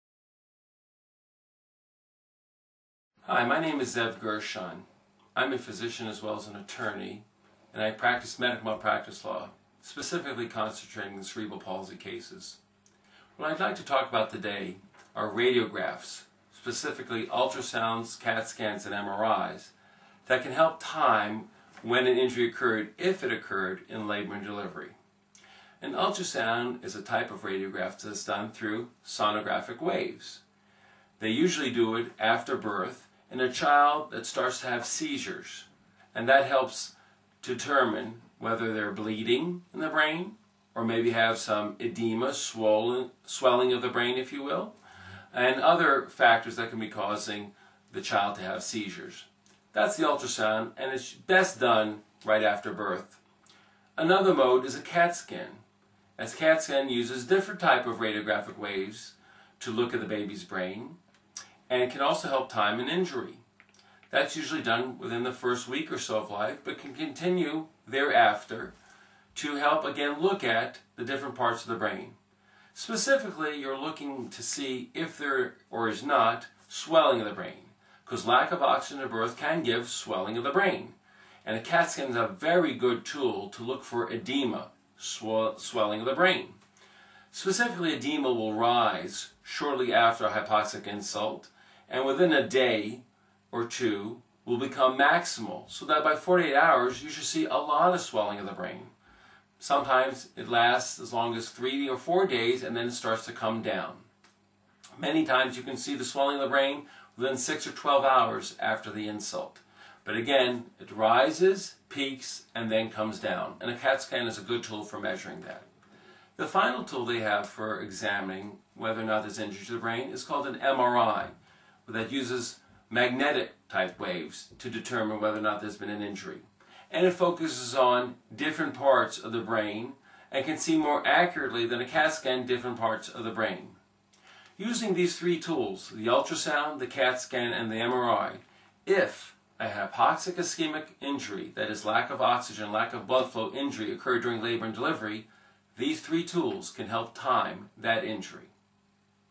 Cerebral Palsy- Radiographs
Zev Gershon MD explains about Ceribral Palsy and the different radiographs used after birth such as ultrasound, cat scan, MRI